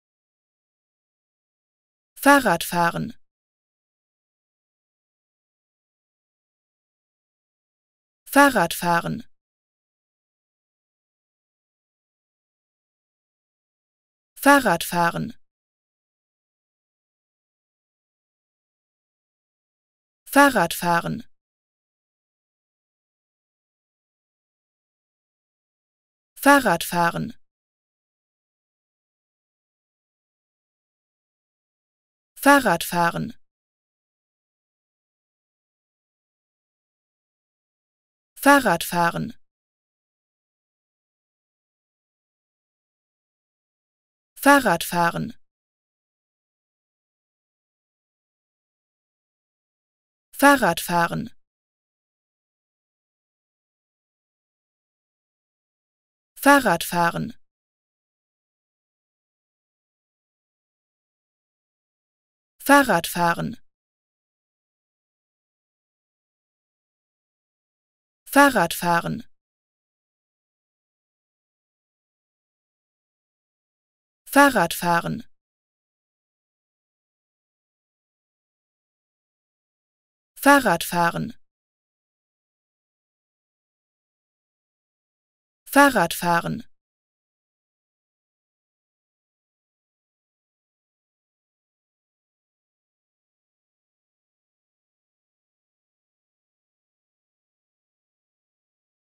How to Pronounce ''Fahrrad fahren'' (ride a bike) Correctly in German
Learn how to say ''Fahrrad fahren'' in German with this short pronunciation tutorial.